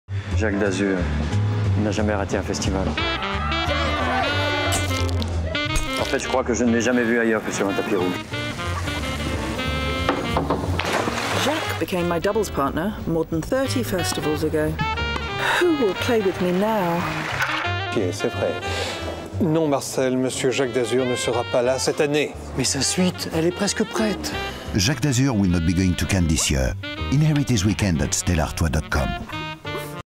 Could you be Jacques Heir to his Cannes suite?
Jacques D'Azur, missing presumed dead: last location, an island populated by beautiful women.

Stella 4% are searching for his heir so his Cannes 2010 suite doesn't go to waste. Could it be you?

or